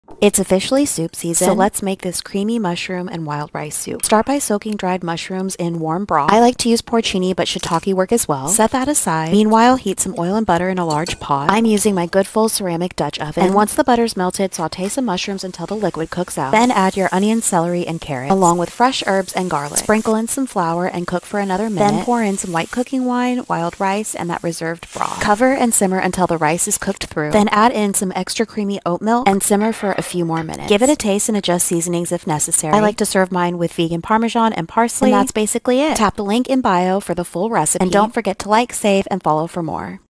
Mushroom Wild Rice Soup
Let's make creamy Mushroom Wild Rice Soup in my Goodful Dutch Oven @eatfigsnotpigs

Link to Goodful Dutch Oven!